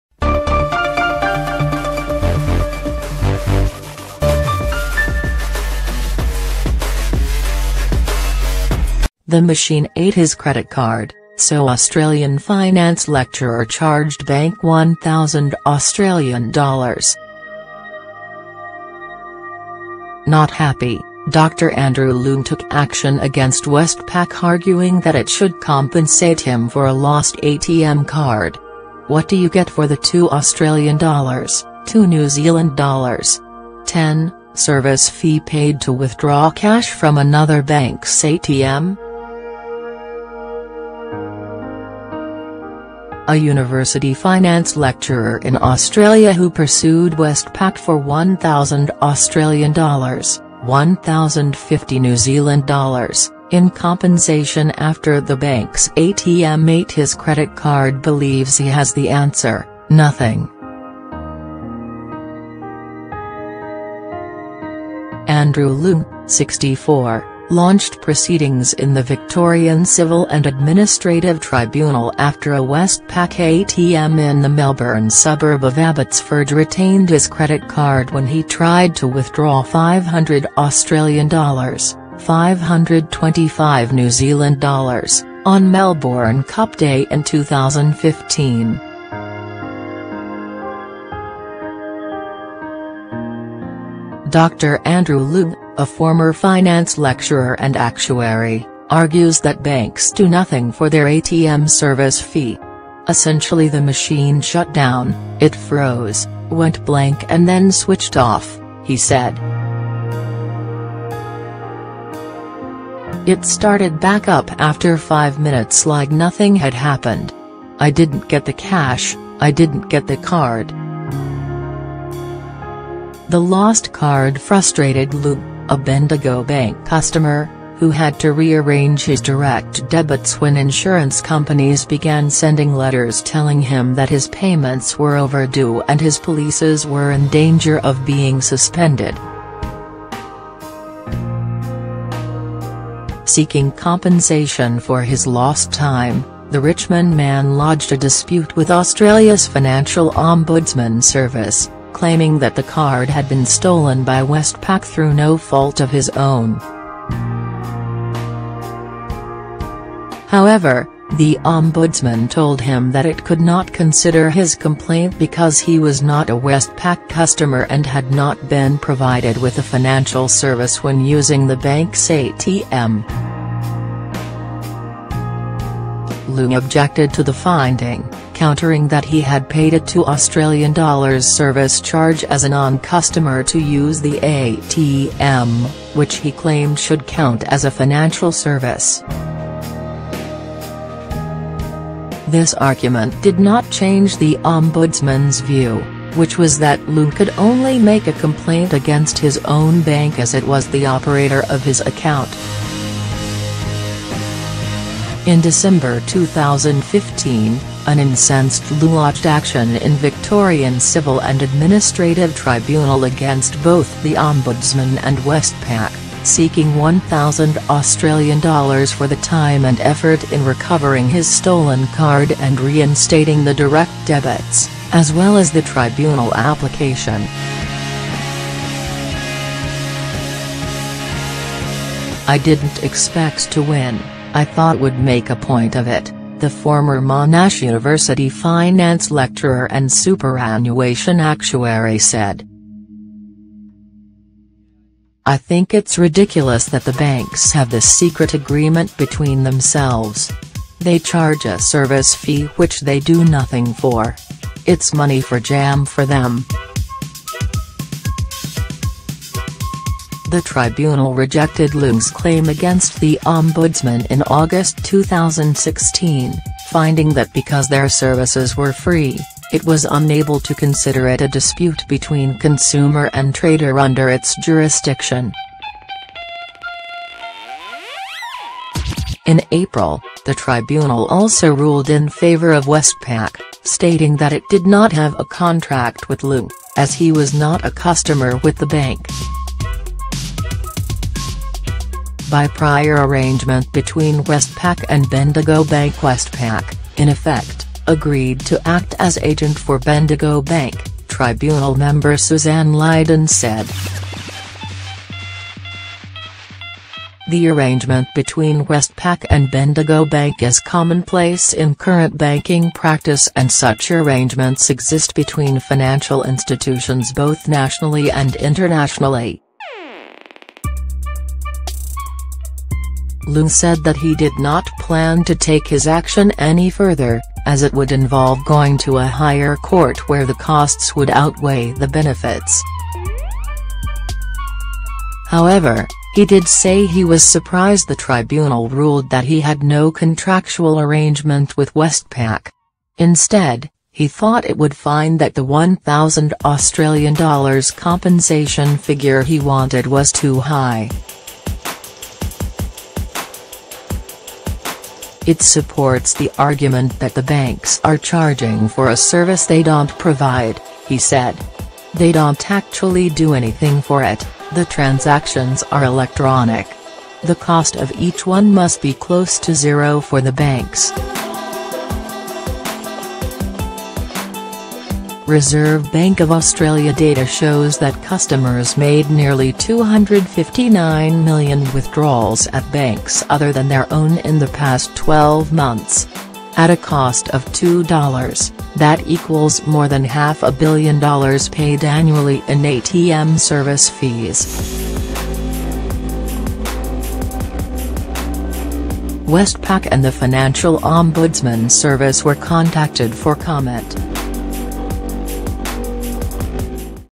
The machine ate his credit card, so Australian finance lecturer charged bank A$1000
NEWS DAYS 24H: The machine ate his credit card, so Australian finance lecturer charged bank A$1000